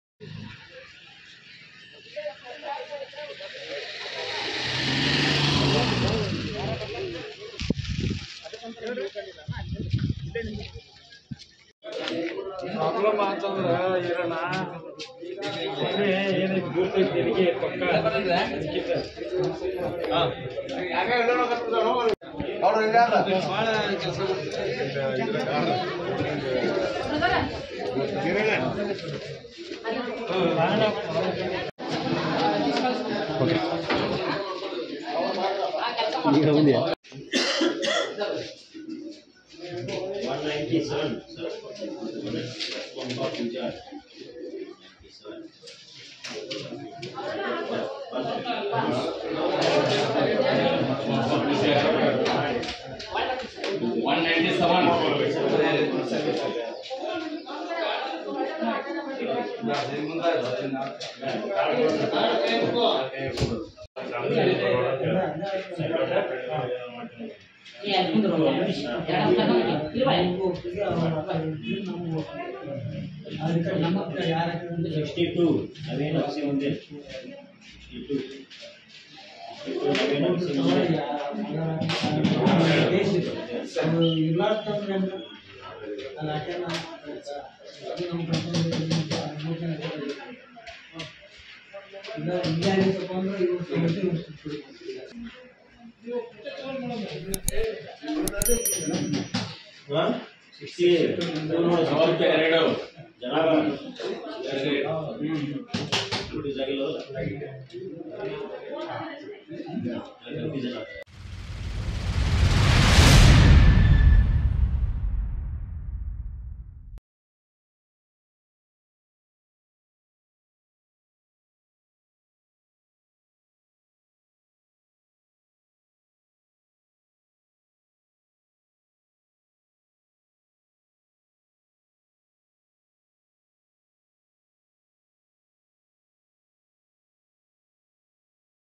Actor Naveen Shankar Votes At PKPS Election | ಪಿಕೆಪಿಎಸ್ ಚುನಾವಣೆಯಲ್ಲಿ ನಟ ನವೀನ್ ಶಂಕರ್ ಮತದಾನ | BAGALKOT
Actor Naveen Shankar Votes At PKPS Election | ಪಿಕೆಪಿಎಸ್ ಚುನಾವಣೆಯಲ್ಲಿ ಮತ ಚಲಾಯಿಸಿದ ನಟ ನವೀನ್ ಶಂಕರ್ | BAGALKOT